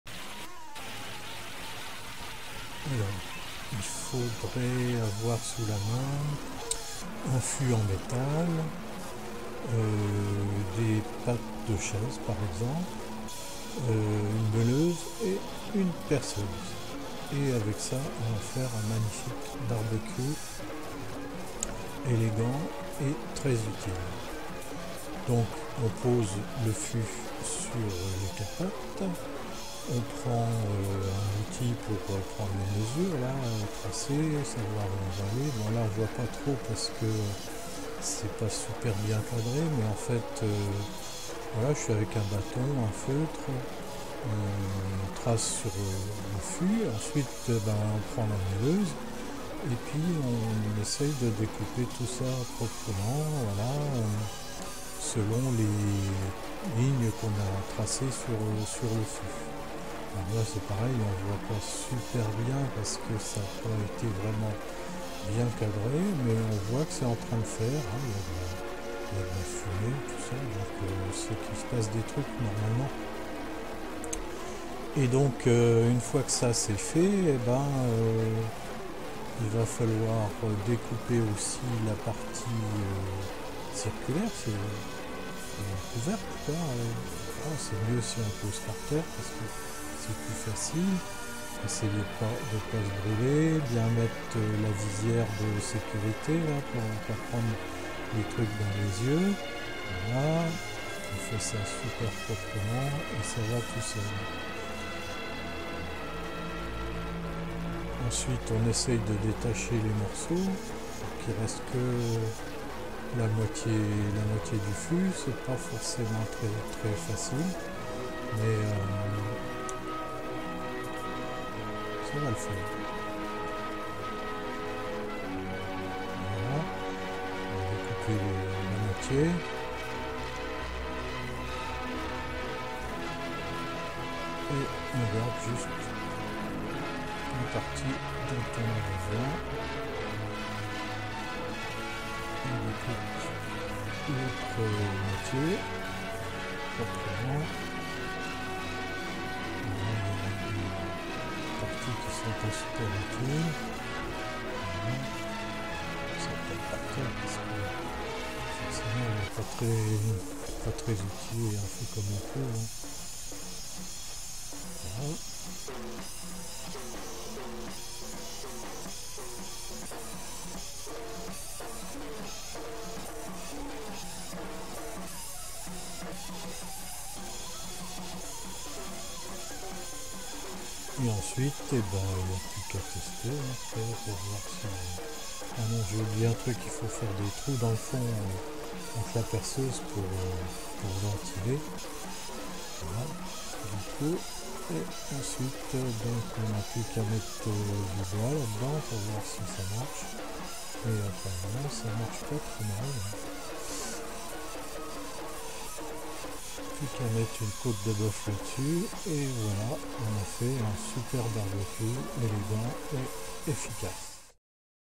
comment fabriquer facilement un barbeku élégant et efficace!
tuto pour fabriquer un barbeku; dans la série "n'achetez plus rien : fabriquez le!"